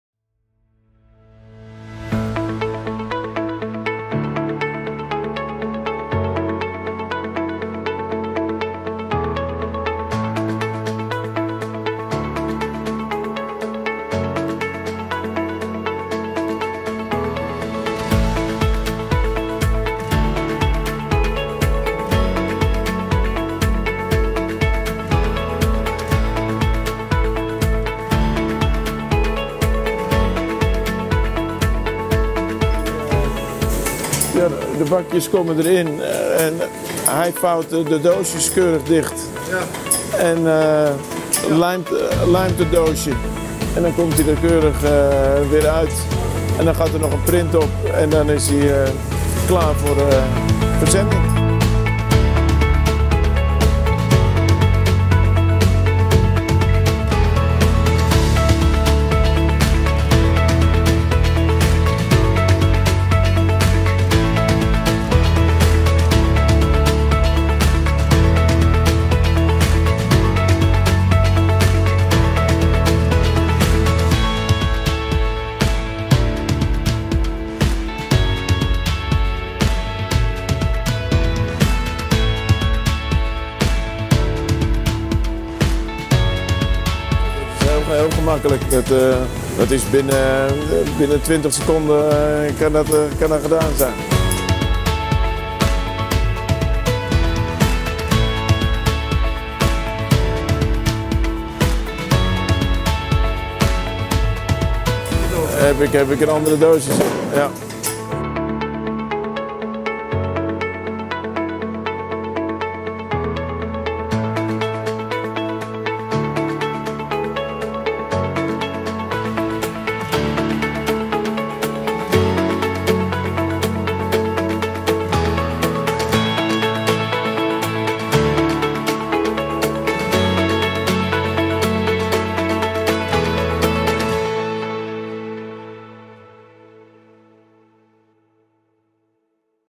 Wraparound casepacker with quick and easy change over
Wraparound casepacker for 18- 20 boxes per minute